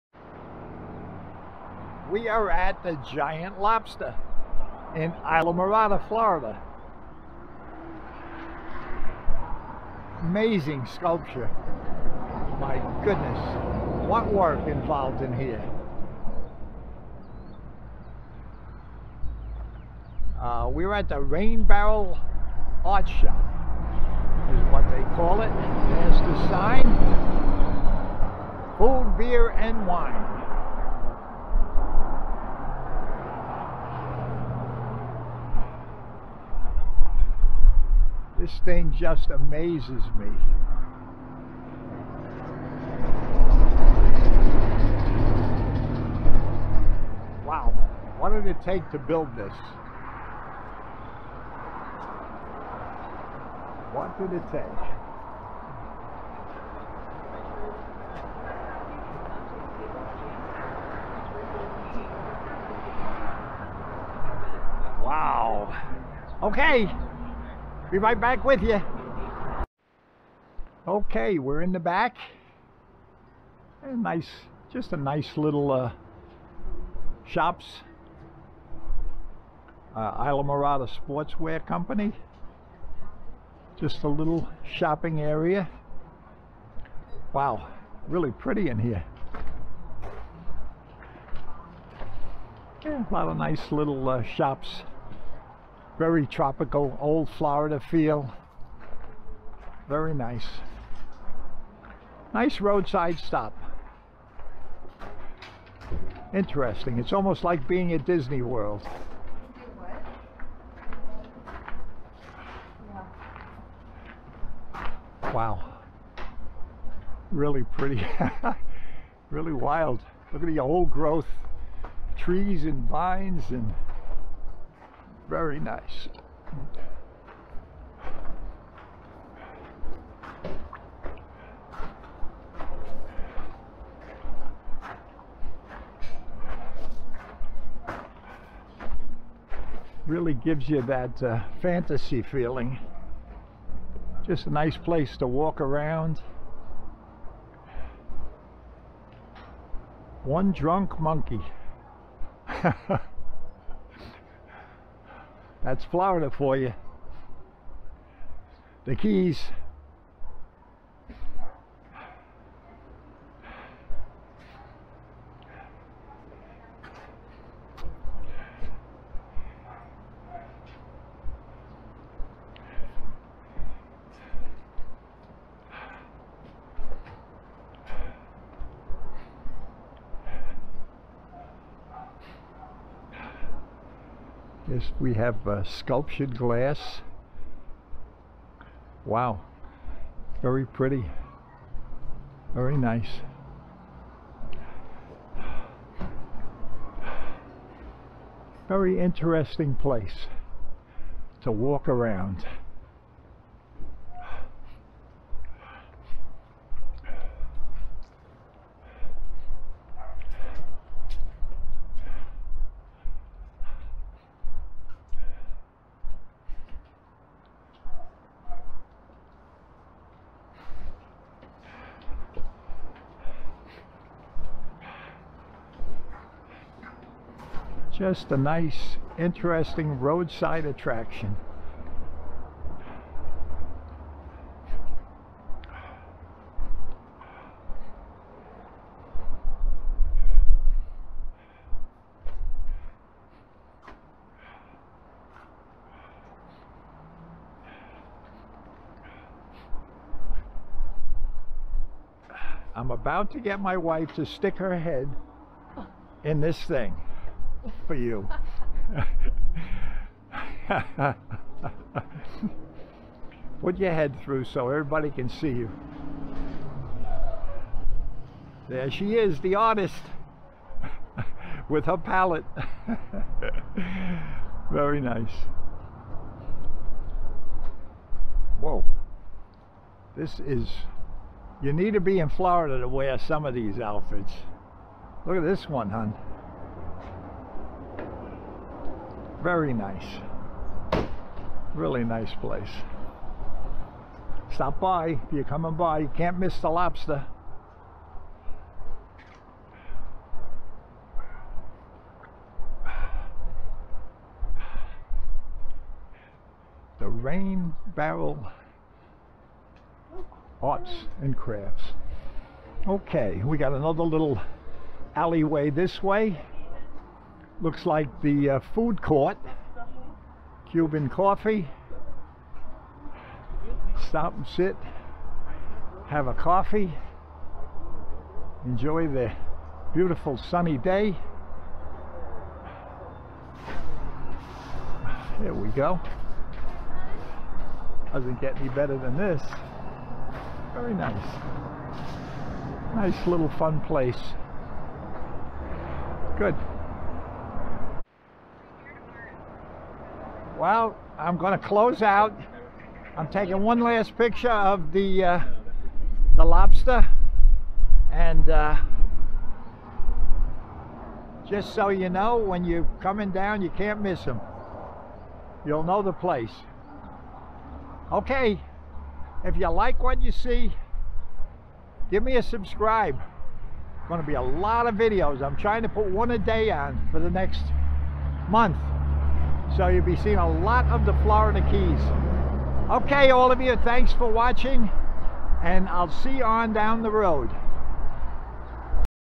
INCREDIBLE ART/ SCULPTURE OF GIANT LOBSTER/ ON US#I SLAMORADA1 FLORIDA KEYS
GREAT ROADSIDE ATTRACTION AND GIFT SHOP, NICE PHOTO OP, WORTH A STOP